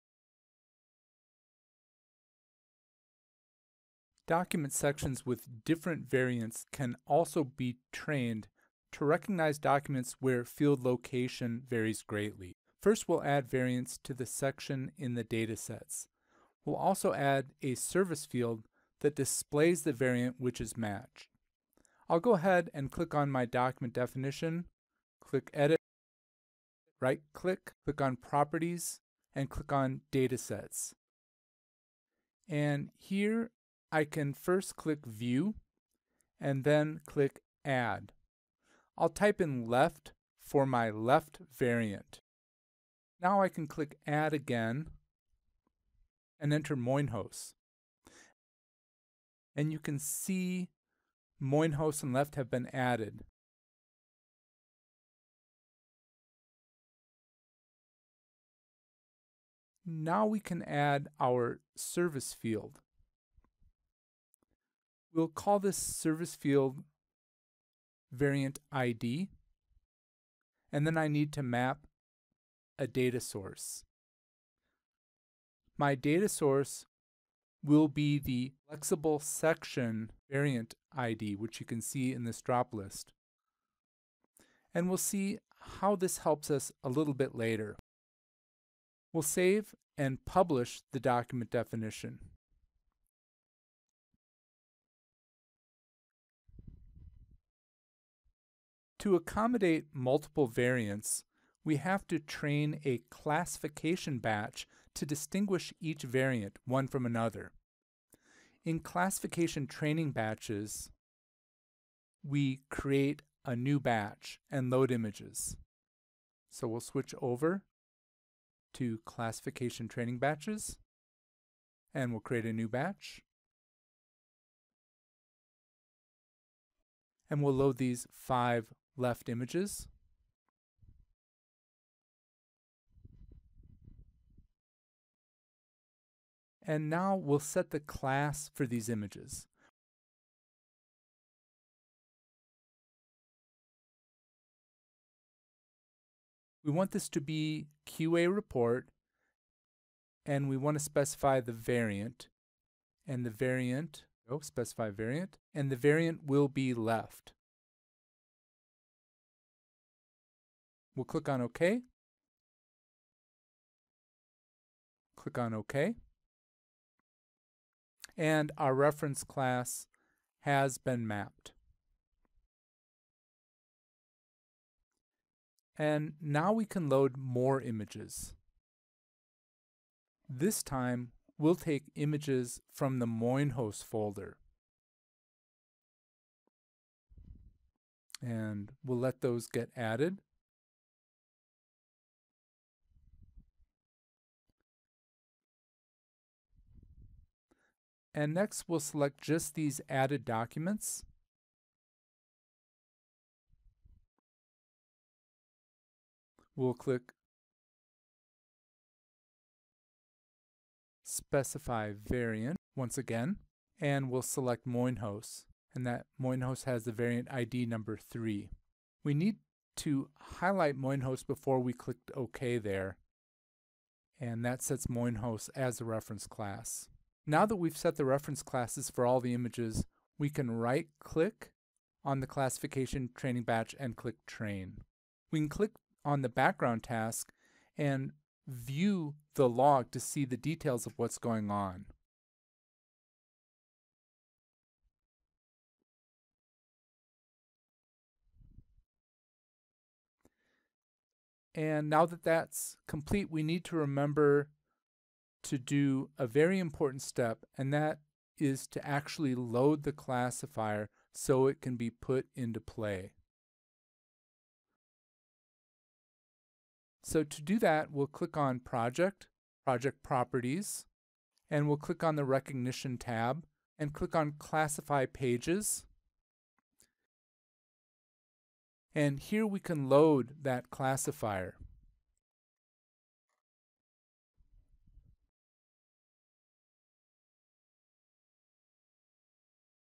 ABBYY FlexiCapture Demo: Field Extraction Training (part 2 of 3)
Watch this demo and learn everything about field extraction in ABBYY FlexiCapture 12. To learn more about FlexiCapture 12, please